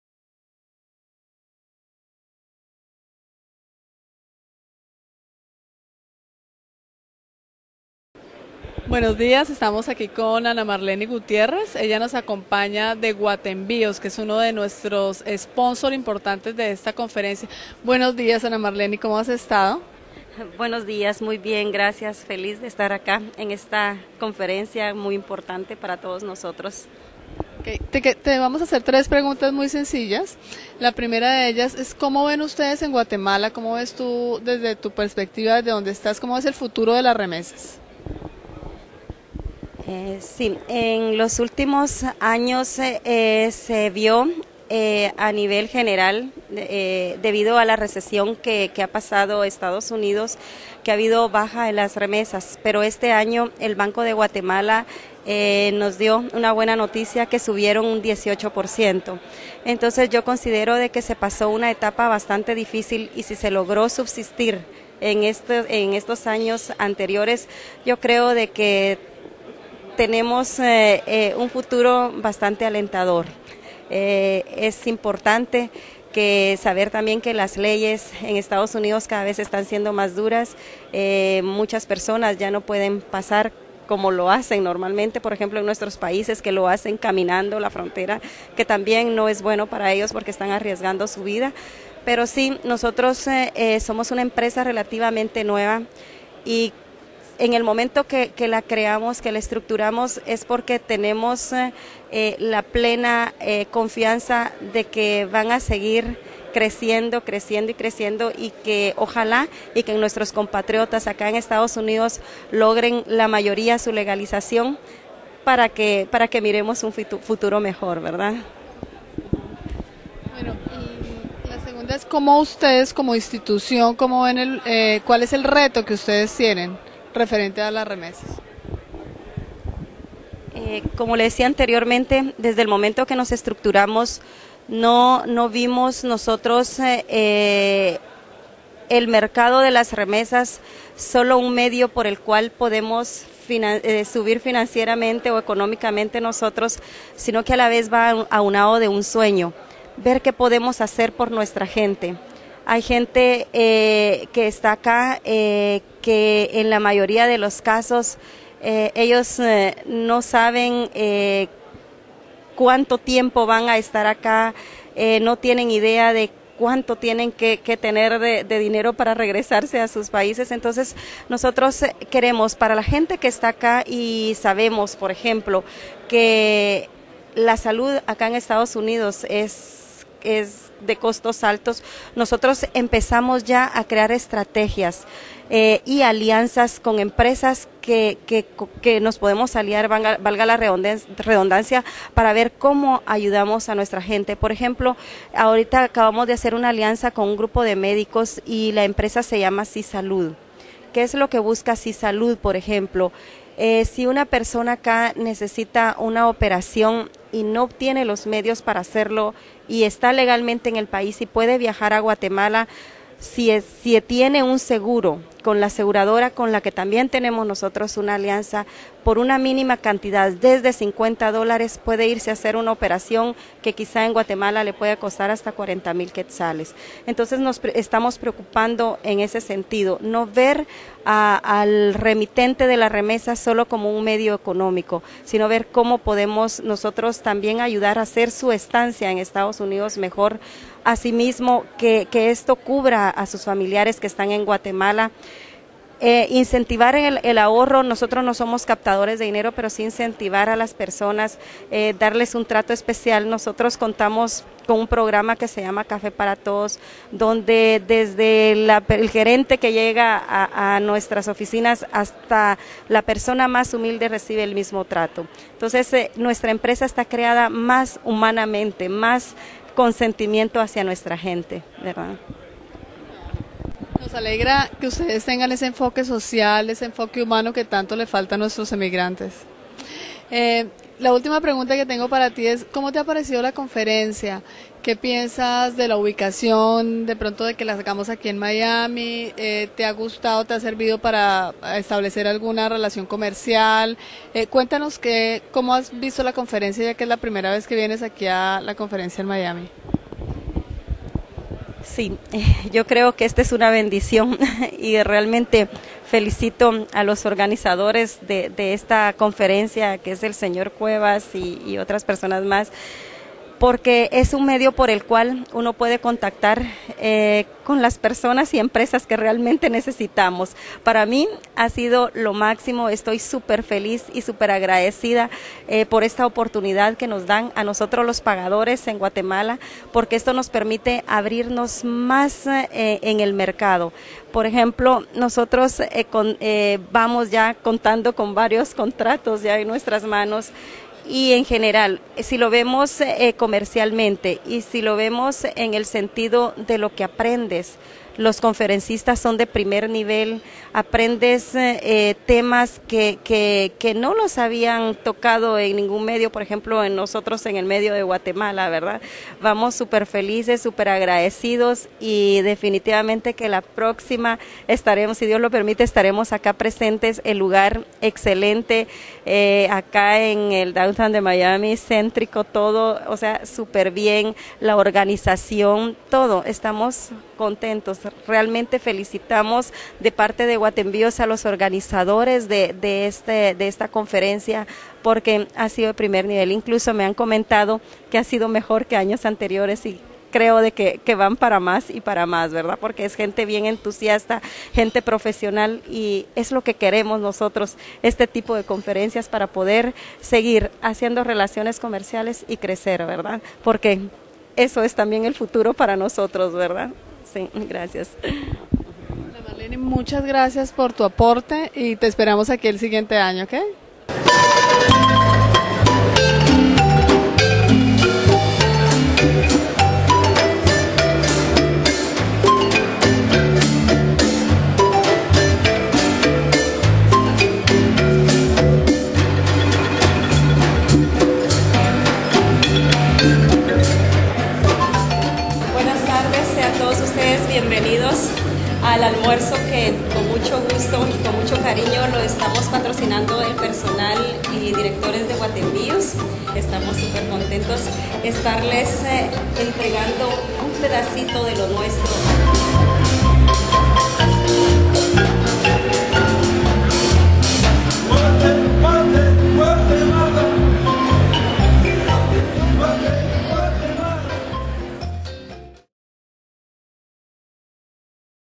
Anamar Gutierrez - IMTC MIAMI 2010 - Entrevista
Entrevista de Zory Muñoz ( IMTC) a Anamar Gutierrez de Guatenvios, empresa de pago y env'io de remesas de Guatemala, sobre el futuro de las remesas y la industria de la transferencia de dinero, los desafios que enfrenta esta industria y sus opiniones sobre la Conferencia realizada en Miami en Octubre de 2010. Ademas tenemos imagenes del almuerzo que patrocinara Guatenvios en la IMTC MIAMI.
La IMTC MIAMI - International Money Remittance Conference - se realiza en Miami cada año desde 2006 en el mes de Octubre y en su versión 2010 convocó a más de 350 participantes de USA, Latinoamérica, el Caribe y otros países. La IMTC es una realizacion de la NMTA (National Money Transmitters Association y Mohr World Consulting
Producción y Direccion: David Landsman y Hugo Cuevas-Mohr
Camaras y Edición: Cuca Pacheco
Entrevista Zory Muñoz.
Miami, Octubre 2010